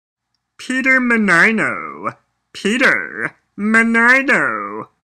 How to Pronounce Peter Mannino Winnipeg Jets NHL Hockey Player Runforthecube
Learn the proper way to say and pronounce the name Peter Mannino hockey player for the NHL Winnipeg Jets in English. Hockey Fight Shootout Goal Hit. Pronounce and say English words correctly. English and American pronunciation.